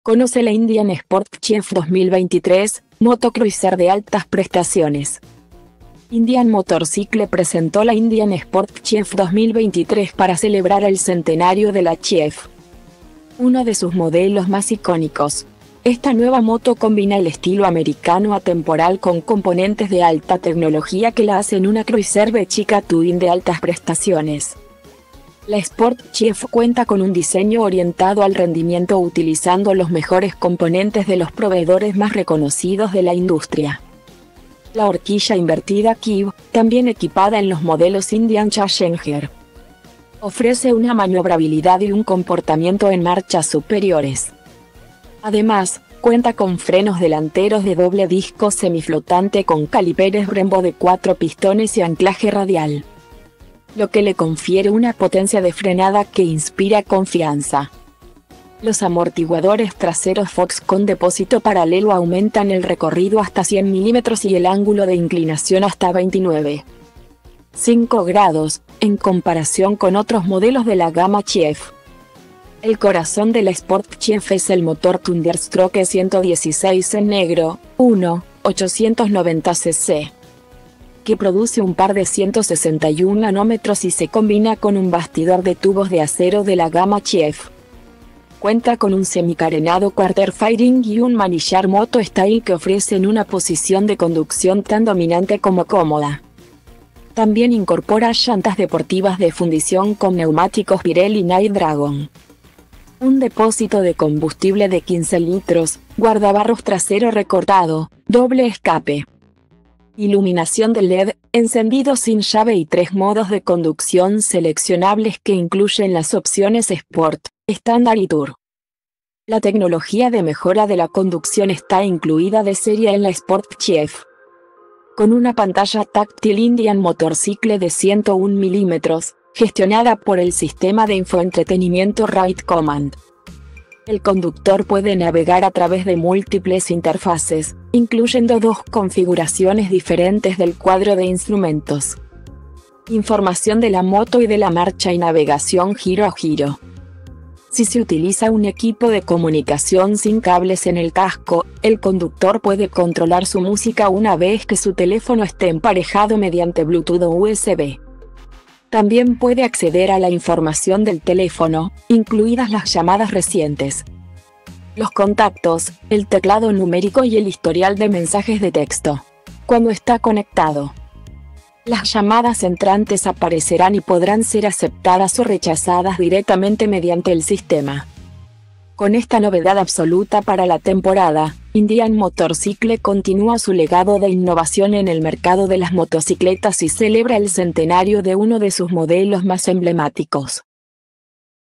Conoce la Indian Sport Chief 2023, moto cruiser de altas prestaciones
Indian Motorcycle presentó la Indian Sport Chief 2023 para celebrar el centenario de la Chief,uno de sus modelos más icónicos. Esta nueva moto combina el estilo americano atemporal con componentes de alta tecnología que la hacen una cruiser V-Twin de altas prestaciones. La Sport Chief cuenta con un diseño orientado al rendimiento utilizando los mejores componentes de los proveedores más reconocidos de la industria.